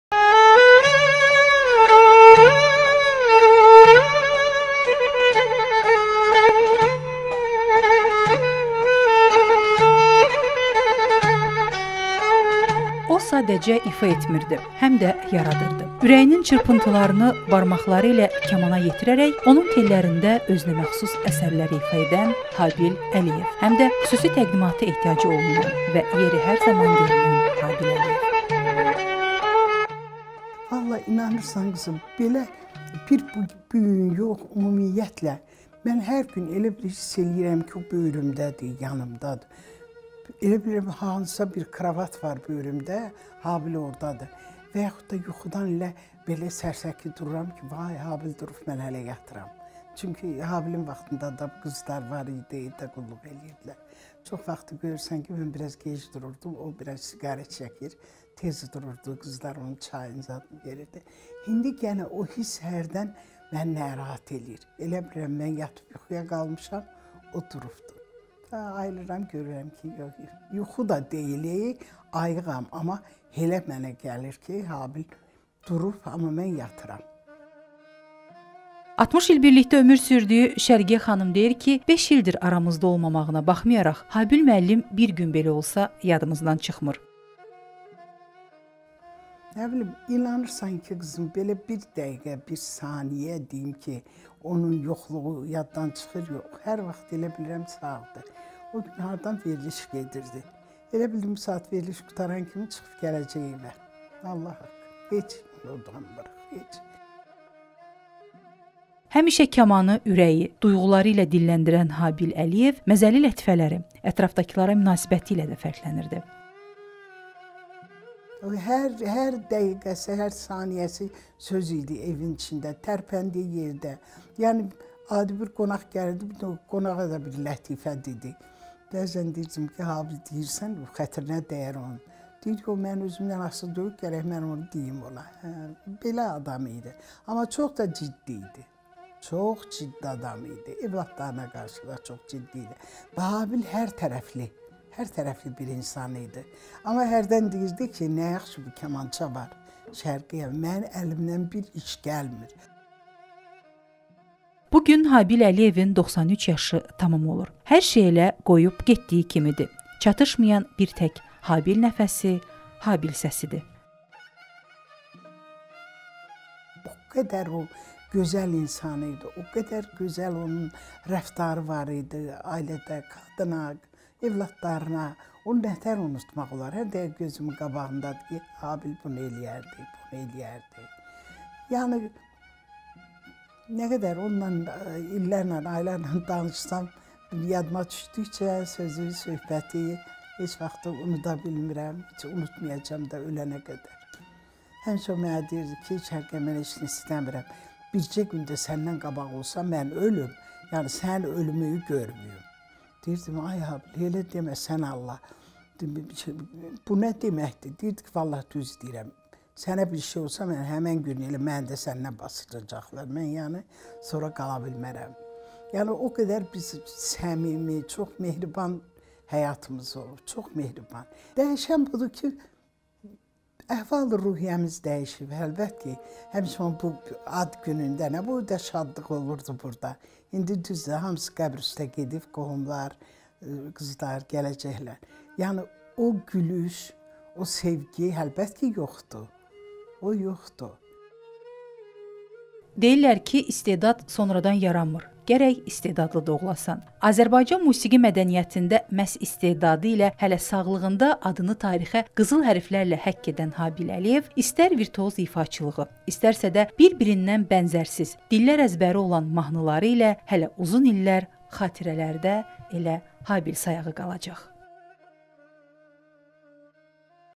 Kamanın virtuoz ifaçısı
Xalq artisti Habil Əliyevin doğumundan 93 il ötür